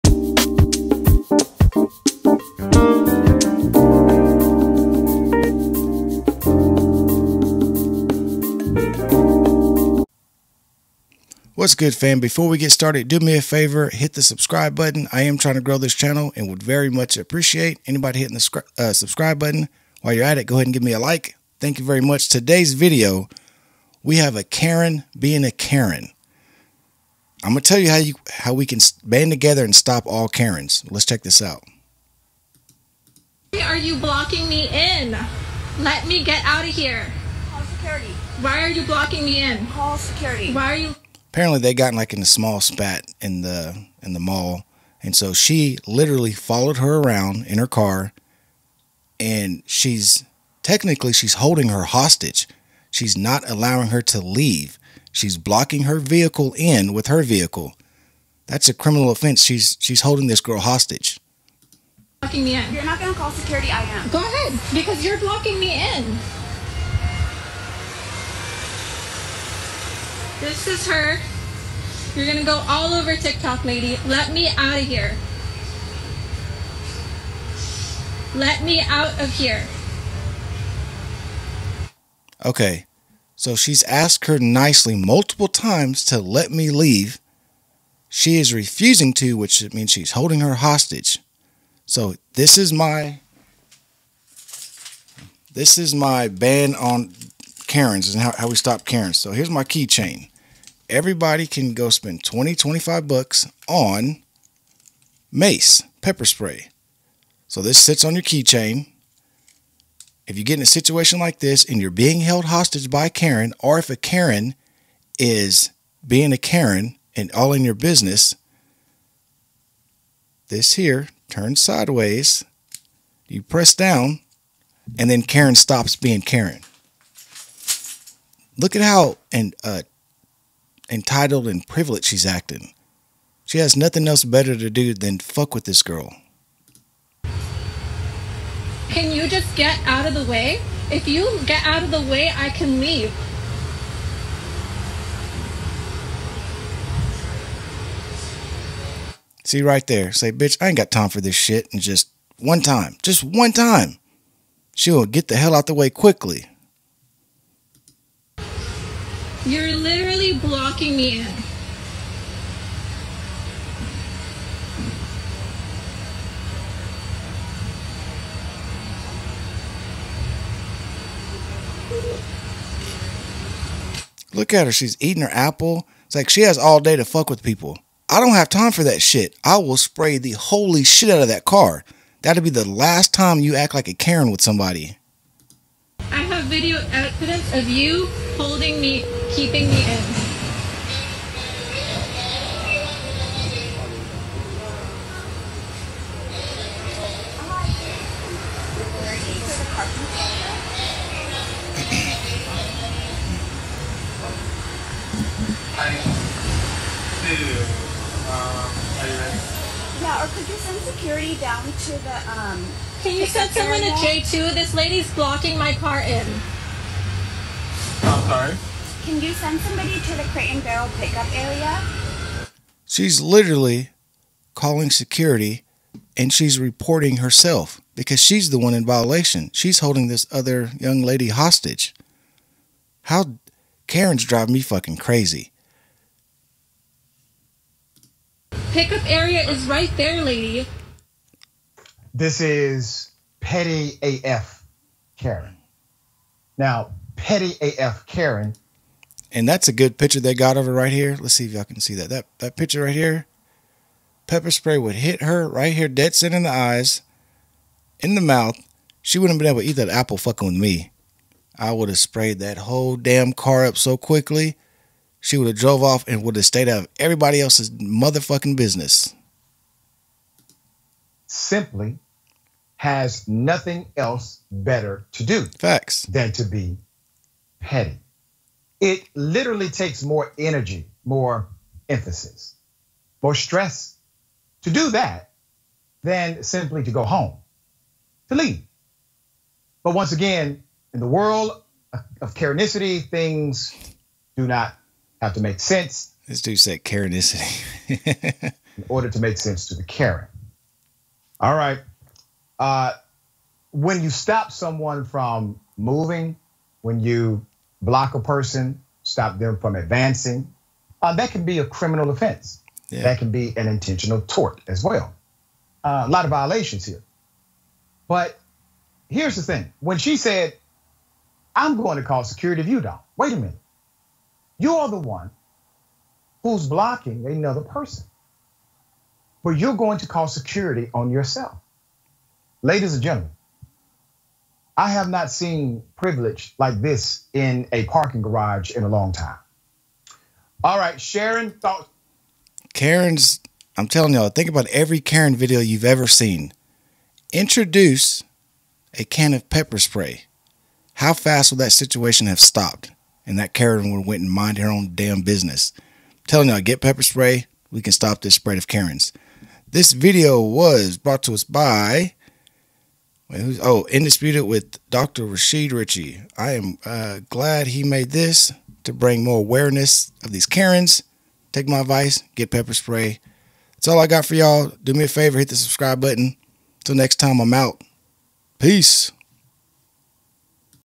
Karen Acting Petty AF Holding Woman Hostage
Karen Acting Petty AF Holding Woman Hostage In A Parking Garage